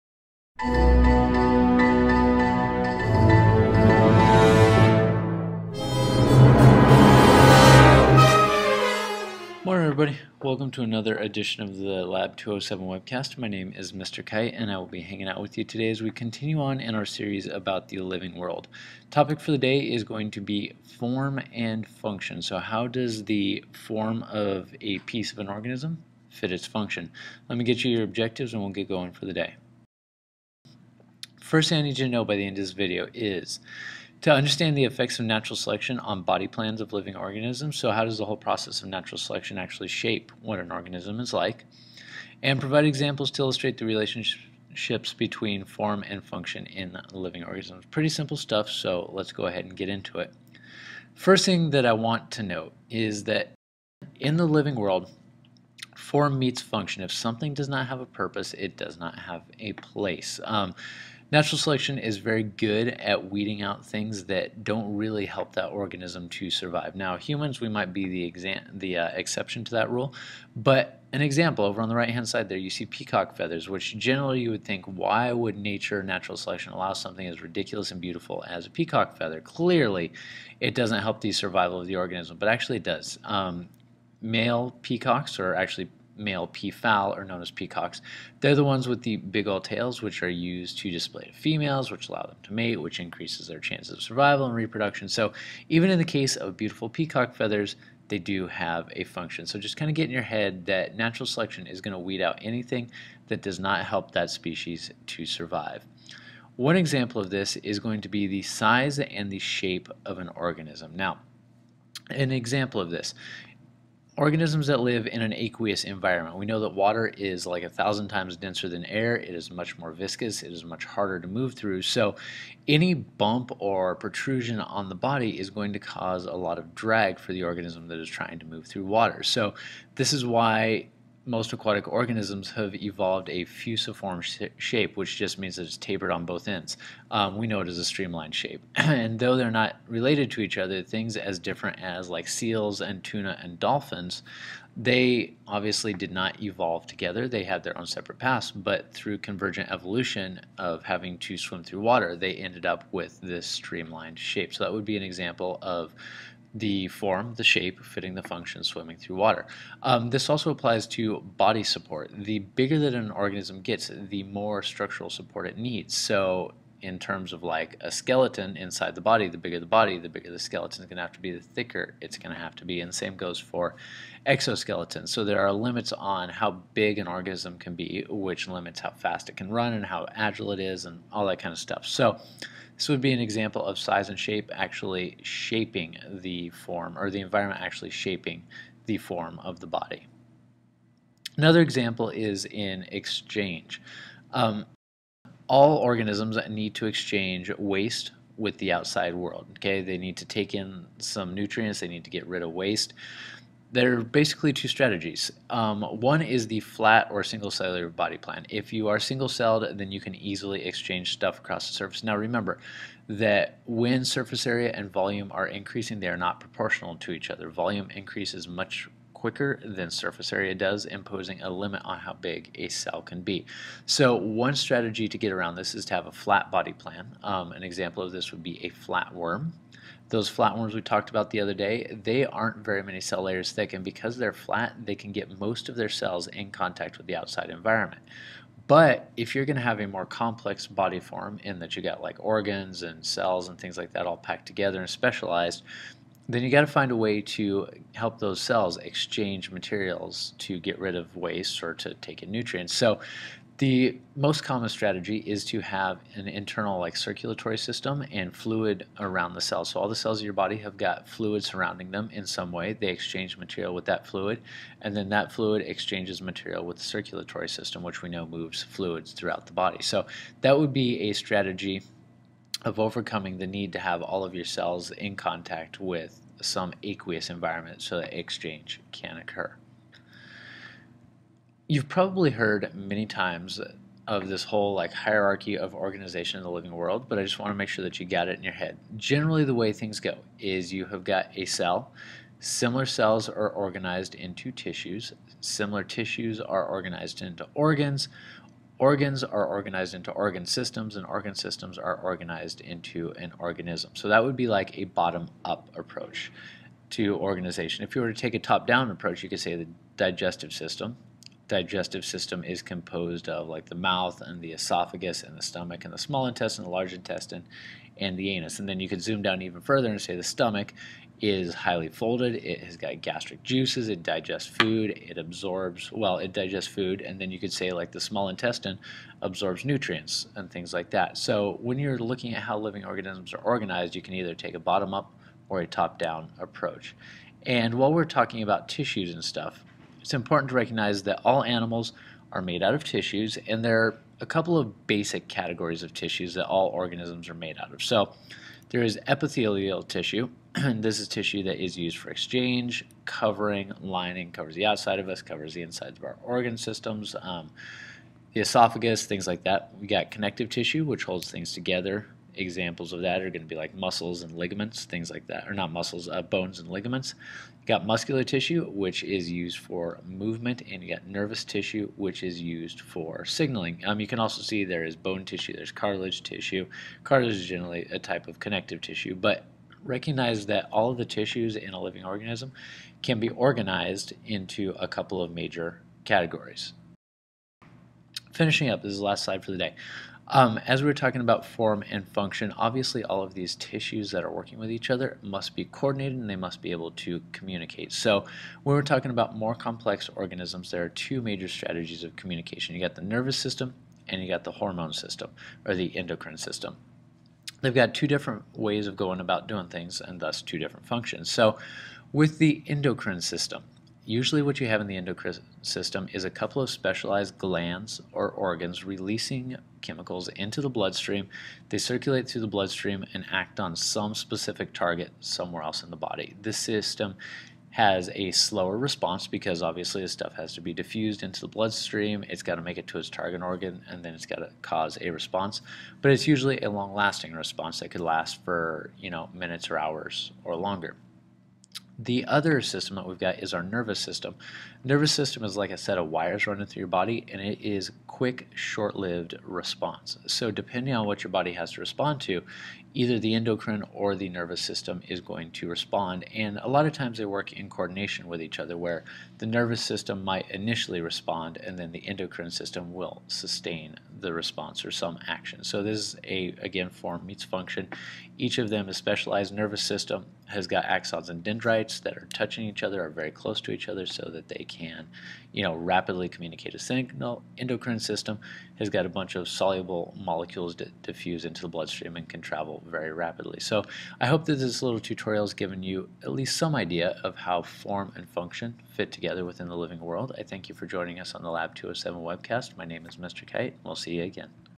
Form and Function
An overview of the connection between form and function in the living world.

Table of Contents:

00:30 - Objectives
00:51 - Form Meets Function
01:46 - Size and Shape
03:10 - Exchange
04:40 - Organization
05:38 - Animal Tissues
06:27 - Coordination and Control